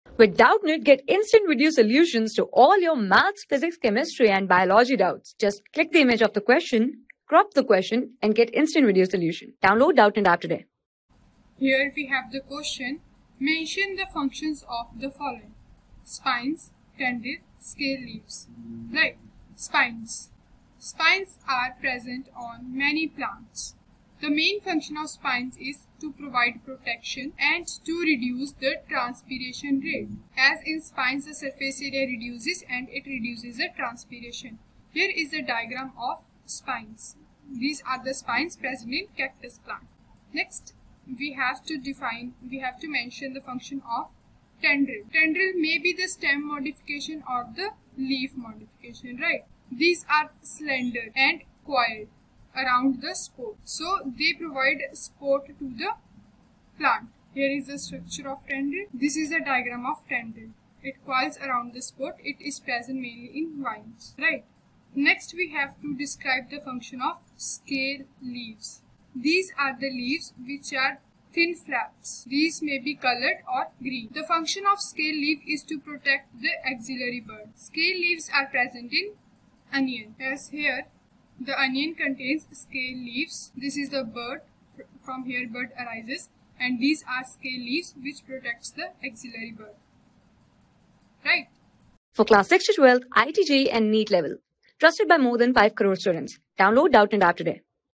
Mention the functions of the following : (i) Spines (ii) Tendril (iii) Scale leaves | 6 | THE L...
Mention the functions of the following : (i) Spines (ii) Tendril (iii) Scale leaves
Class: 6
Subject: BIOLOGY
Chapter: THE LEAF
Board:ICSE

👉 Have Any Query? Ask Us.

Our Telegram Pages:
class 6
6 class ka math syllabus